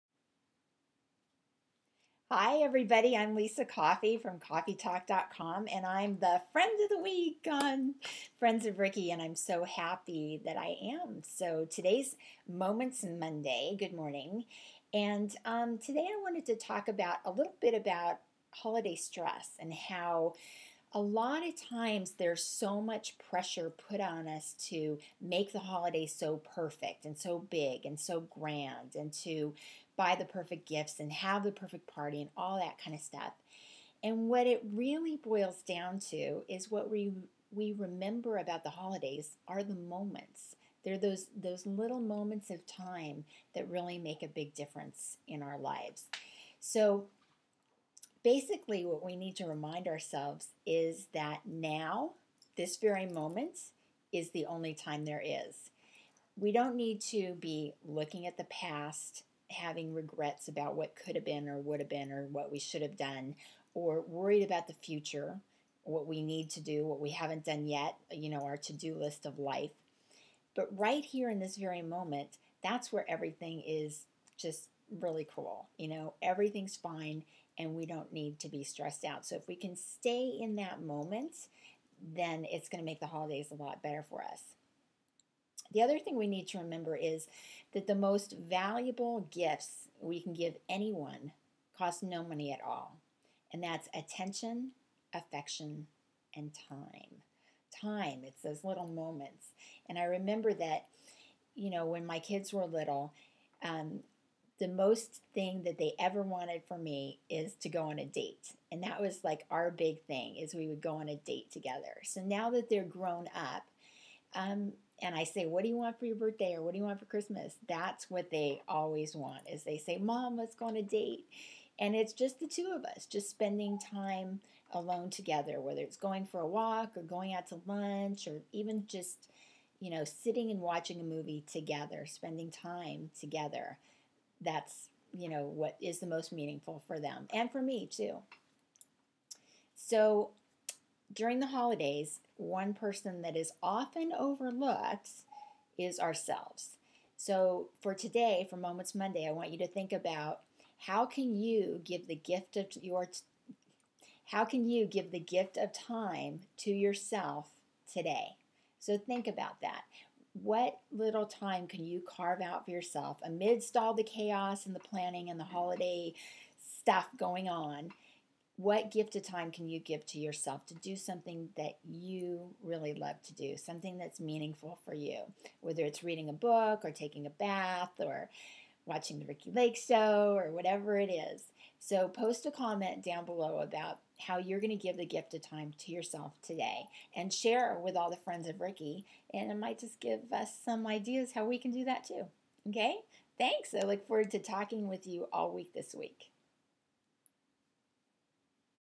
Moments Monday
Lifestyle and relationship expert Lissa Coffey talks about Moments Monday for Friends of Ricki, the facebook page for The Ricki Lake Show.